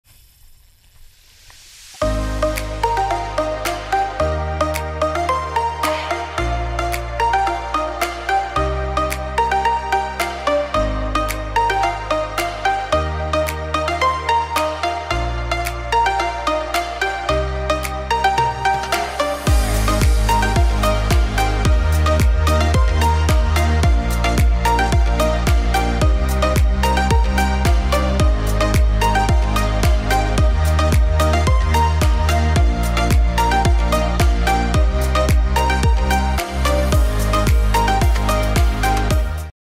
Diamond Trevalley - At Pasir Ris Park fishing in Singapore
Hooked on this Diamond Trevalley at Pasir Ris equipment use as followed

Tackle info:

Reel: Shimano Stradic Ci4+ C200M Yumeya
Rod: Abu Garcia salty stage KRX Ajing Custom
Lure: Zerek chili padi 10g
Line: Sunline siglon